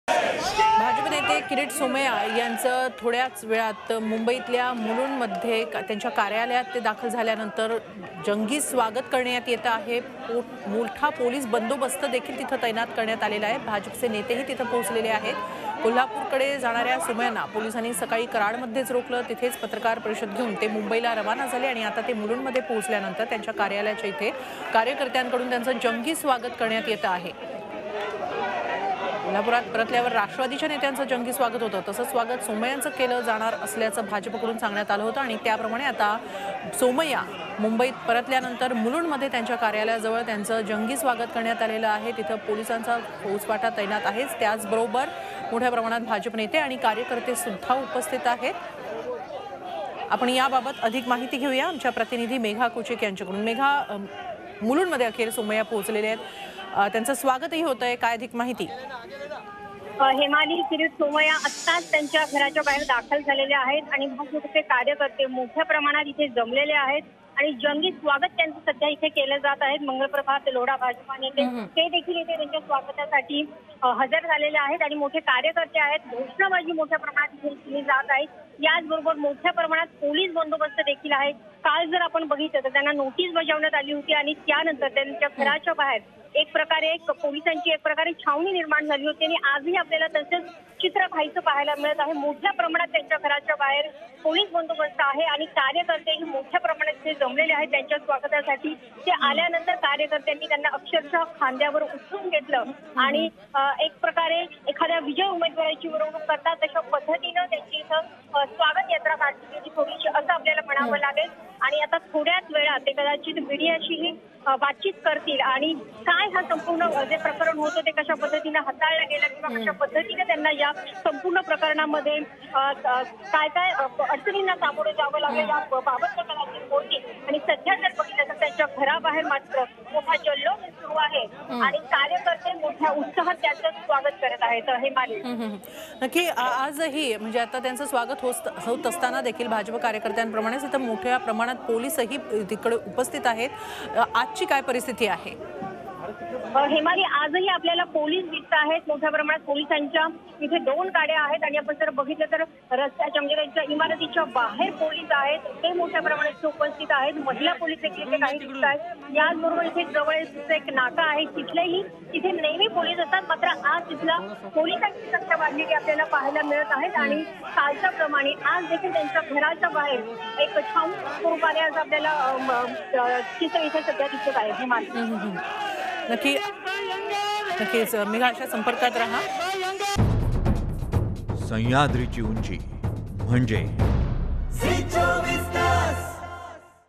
जेंव्हा सोमय्या कराडवरून परत येतात.. । Mulund BJP Leader Kirit Somaiya Grand welcome In Mumbai
जेंव्हा सोमय्या कोल्हापूरवरून परत येतात । Mulund BJP Leader Kirit Somaiya Grand welcome In Mumbai